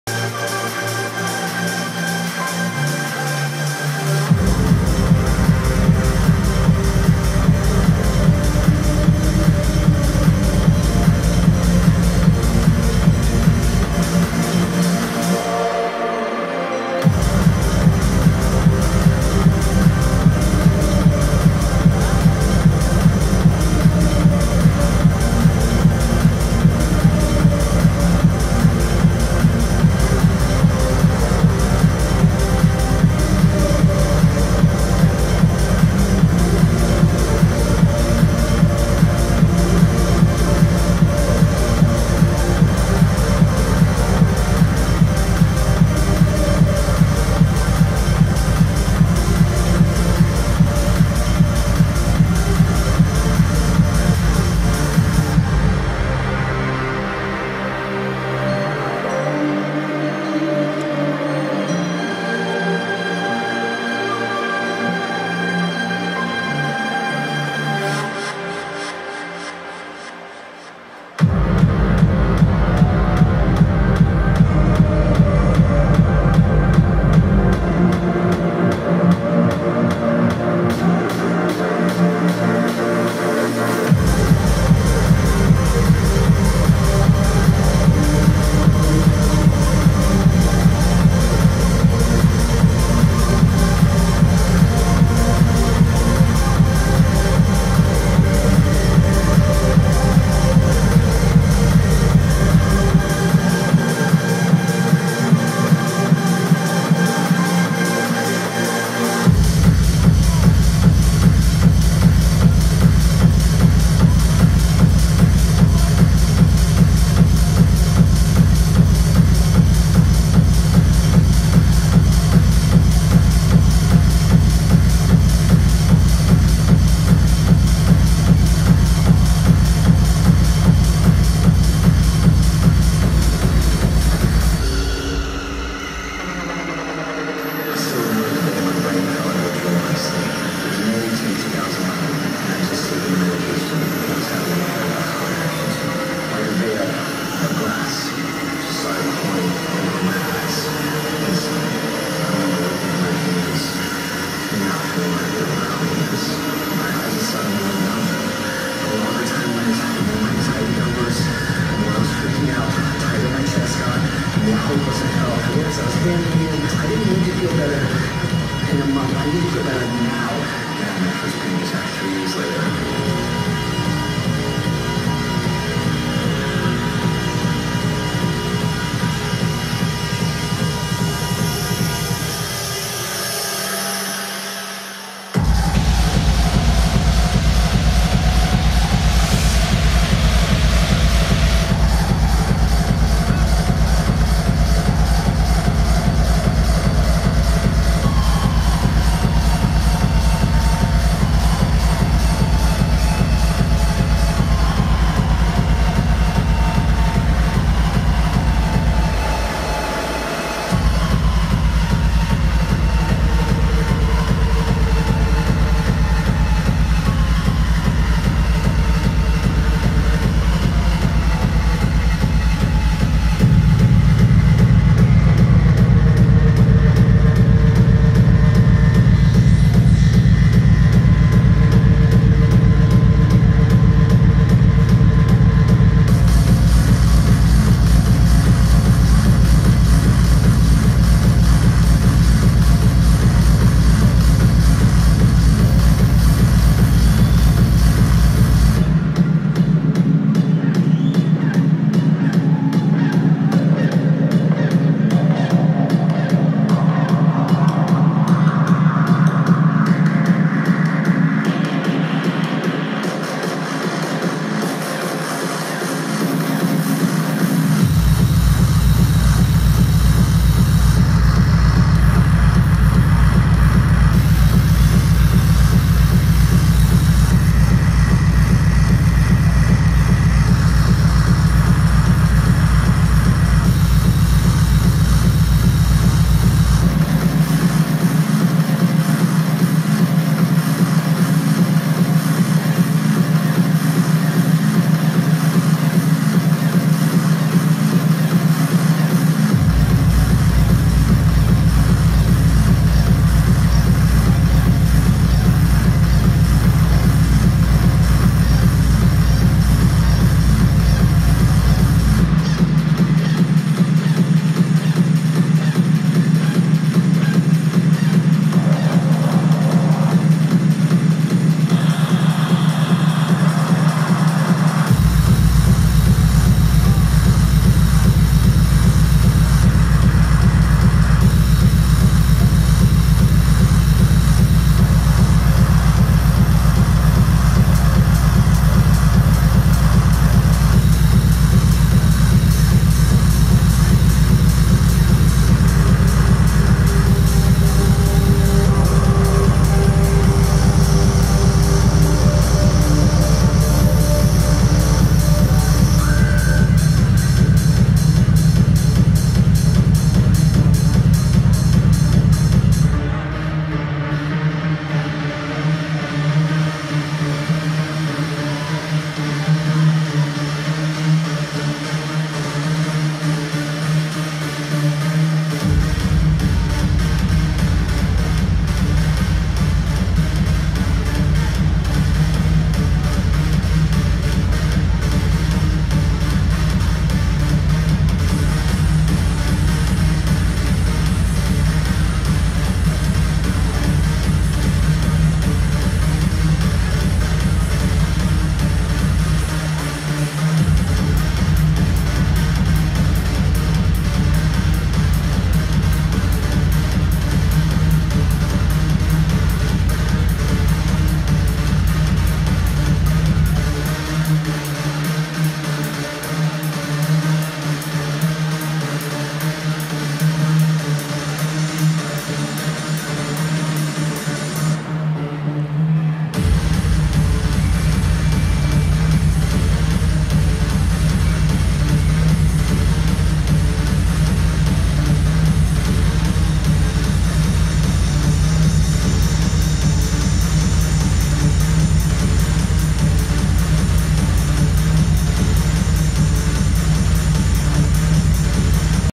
Paula Temple at Awakenings in Gashouder, Amsterdam Dance Event 19 October 2022
Paula Temple at Awakenings in Gashouder, Amsterdam Dance Event 19 October 2022. The video is recorded with an iPhone 12 Pro with a Shure MV88 stereo microphone. Support the artist